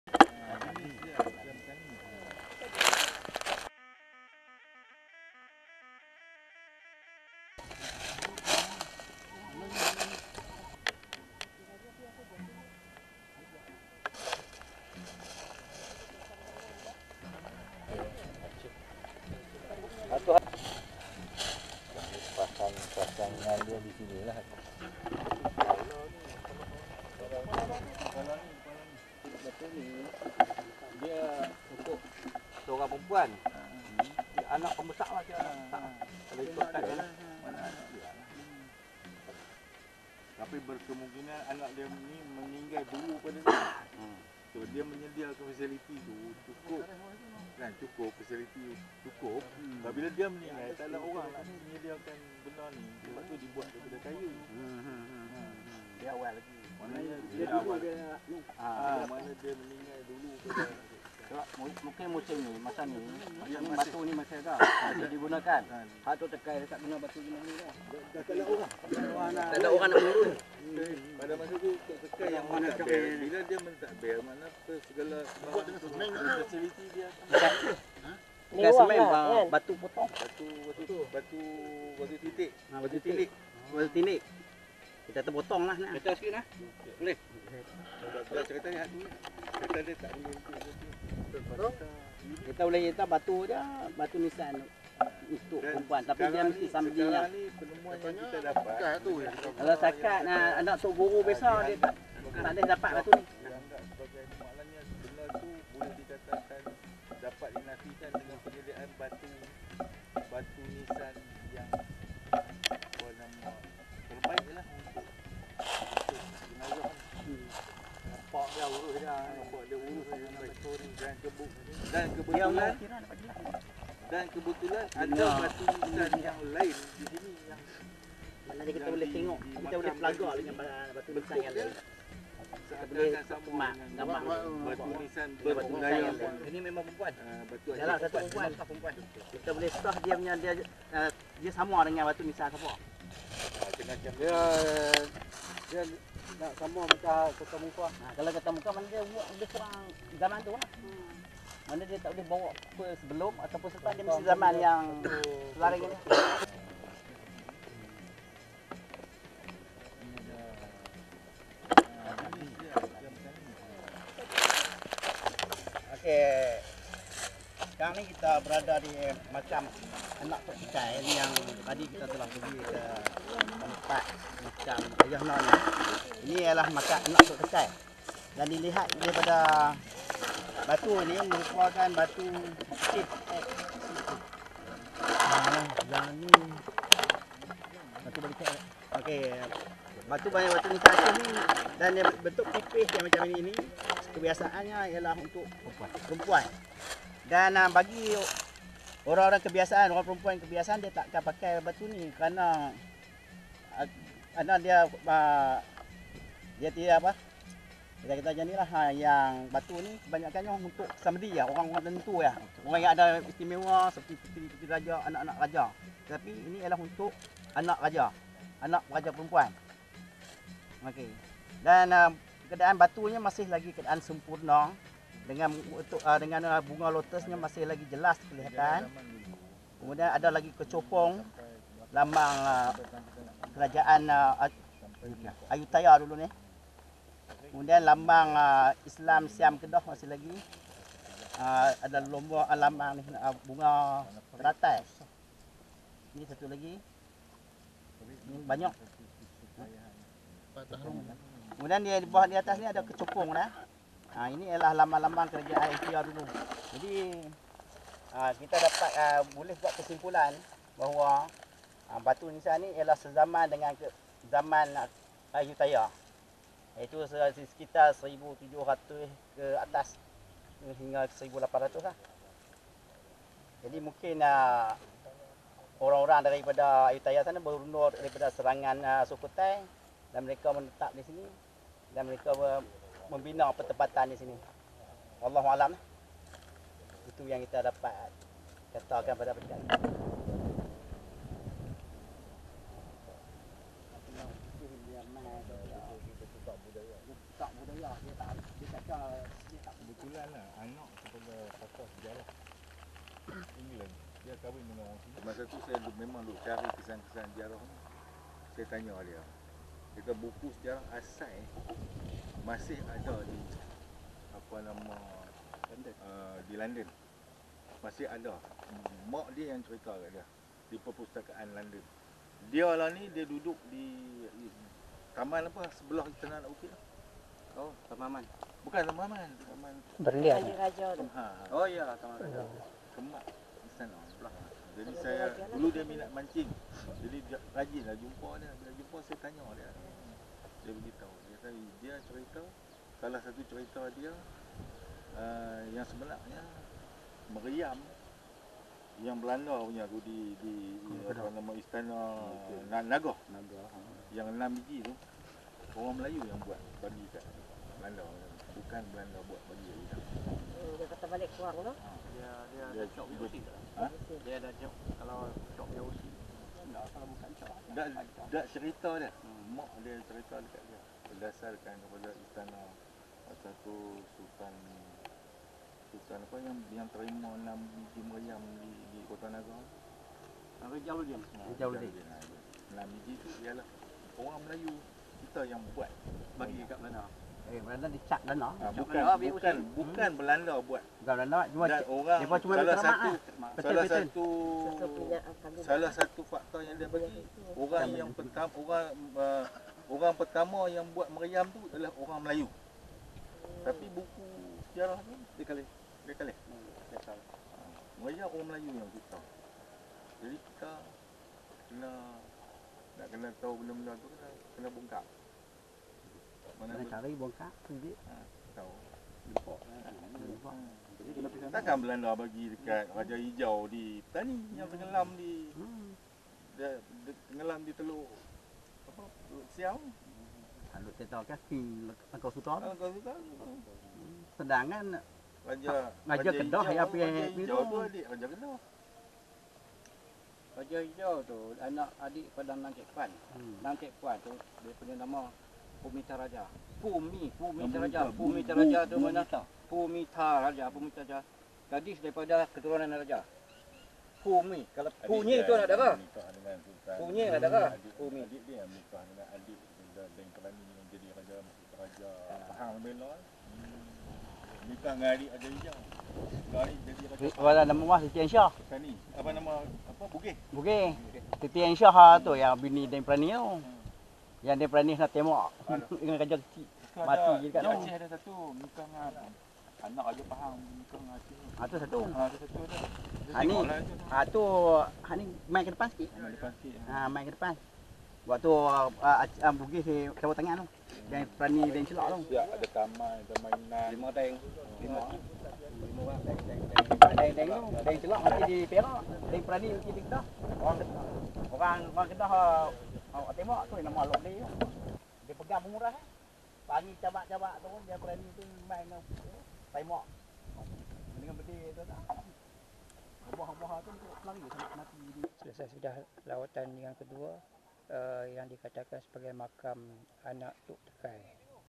MENGAPA MAKAM CHE WAN JAUH DARI MAKAM AYAHANDANYA TOK TEKAI?
Ziarah Makam Che Wan anak kepada Tok Tekai yang dikatakan bermukah dengan seorang lelaki. Che Wan kemudian dihukum mati dan dikuburkan jauh dari tanah perkuburan di mana terletaknya Makam ayahandanya Tok Tekai, seorang ulama yang terkenal pada tahun1800an.

Saya bersama beberapa orang ahli Group Sejarah Realiti Kedah telah berpeluang menziarah makam Che Wan selepas ziarah Makam Tok Tekai.